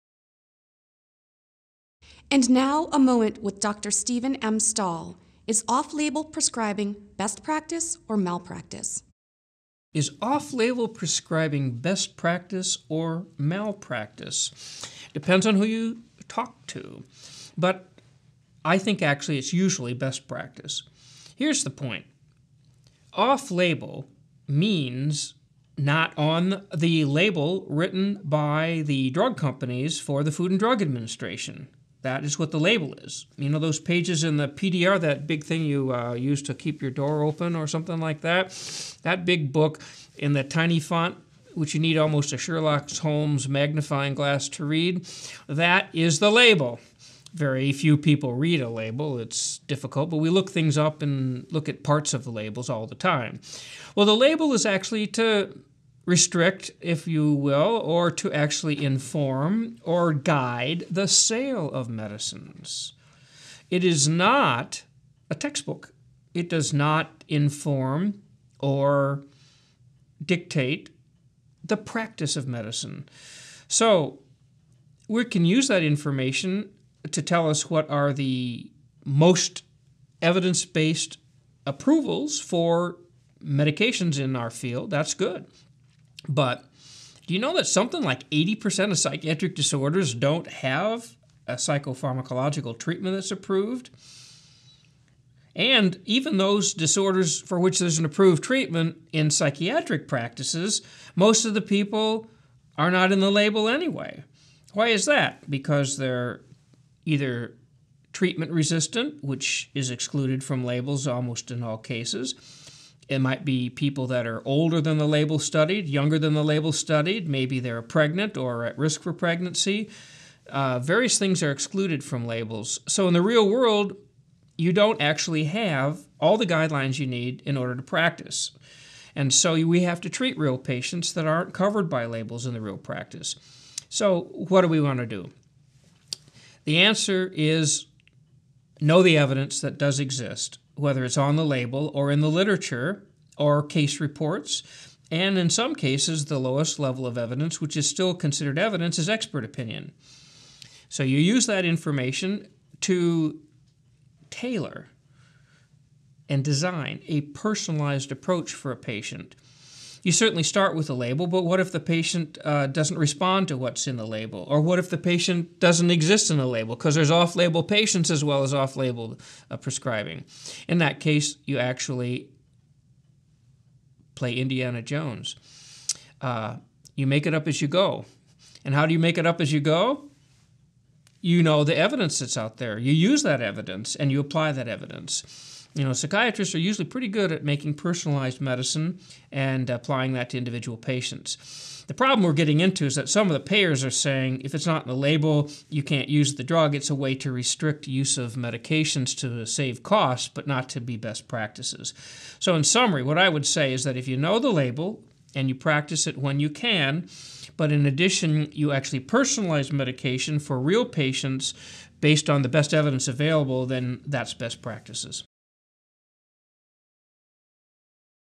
On-label usage, or off-label usage?
Watch as Dr. Stephen M. Stahl discusses on-label and off-label uses of psychopharmacological agents.

Listen to NEI Podcast!